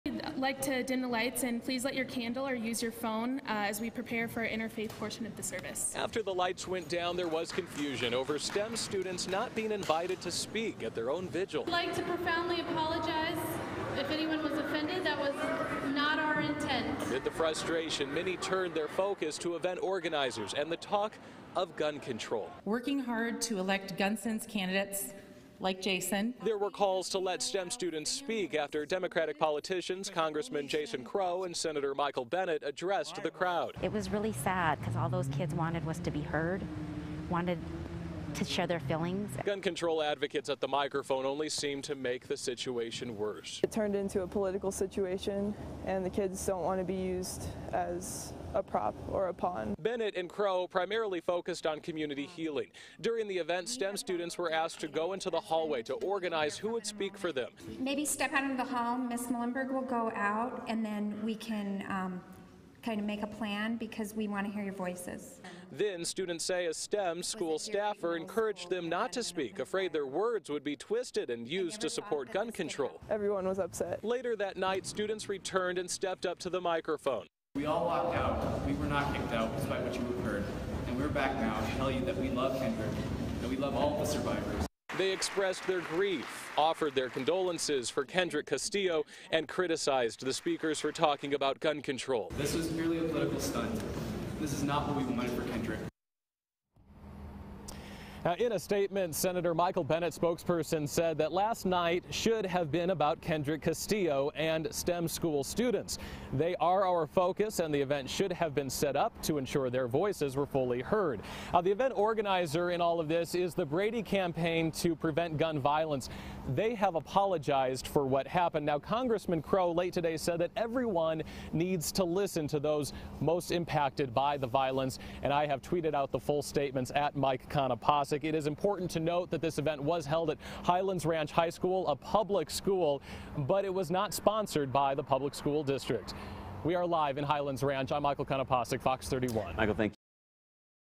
STEM students upset they weren't invited to speak at vigil, frustrated at political tone
A vigil intended to heal a community turned political Wednesday night, leading to a walkout at Highlands Ranch High School. Michael Konopasek reports.